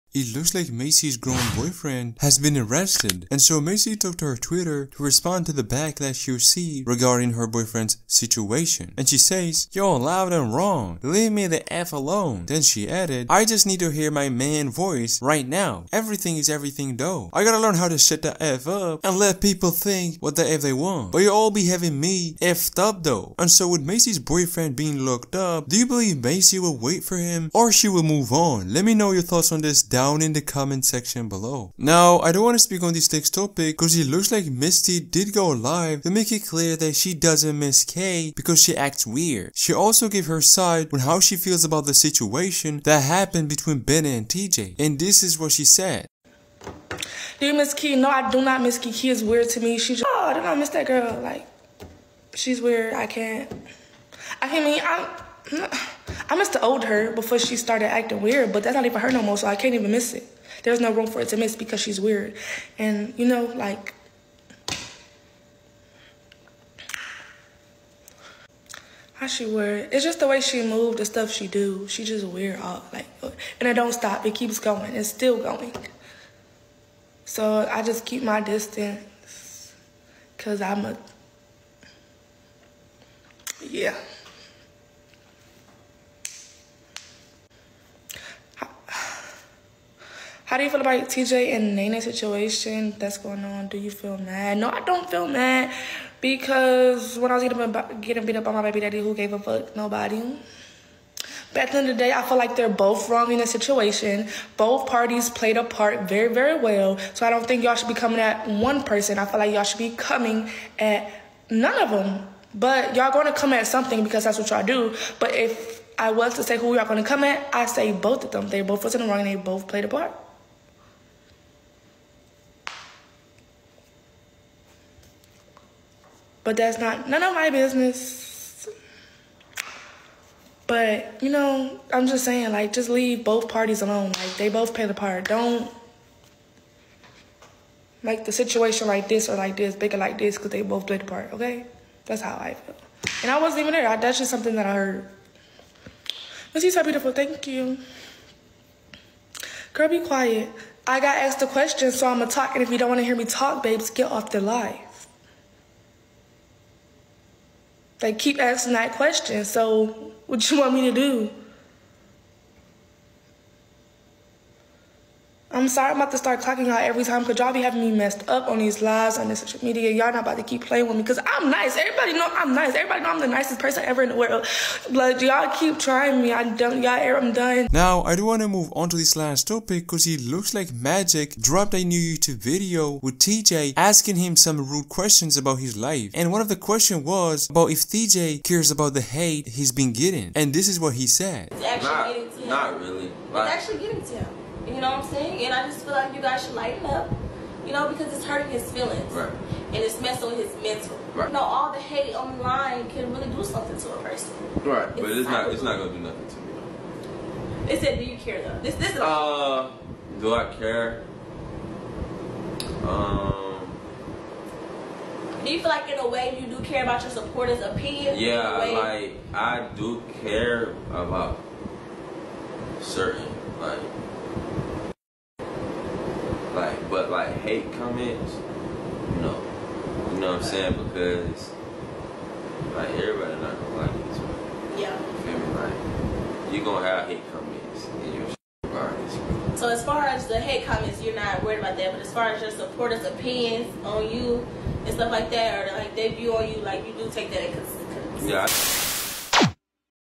Macei's boyfriend got l0cked up 😱 Misty's done with Ke ❤️‍🩹 Magic take on the hate TJ be receiving
Misty is done with Ke ❤️‍🩹 Macei's boyfriend got l0cked up 😱 Magic take on the hate TJ be receiving...